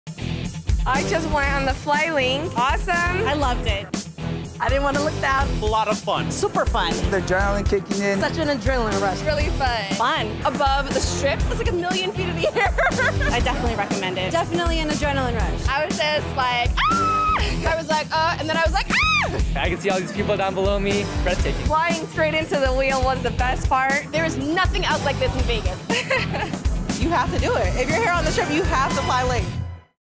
Fly LINQ Zipline Vegas On The Strip | The LINQ Hotel + Experience
The Fly LINQ Zipline Vegas at The LINQ Promenade is the Las Vegas Strip's only zipline experience. Fly at the speed of fun 12 stories above The LINQ Promenade.
You can choose how you want to Fly:
- Fly Seated
- Fly Superhero
- Fly Backwards